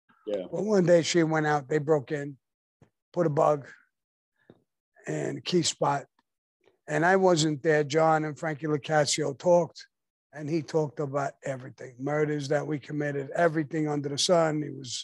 Sammy The Bull Talks About John Gotti And Frankie Loc Being Caught On A Bug
Salvatore "Sammy the Bull" Gravano is an American former mobster who became underboss of the Gambino crime family.

Born: 12 March 1945 (age 77 years), Brooklyn, New York, United States

Grandchild: Karina Seabrook

Children: Karen Gravano, Gerard Gravano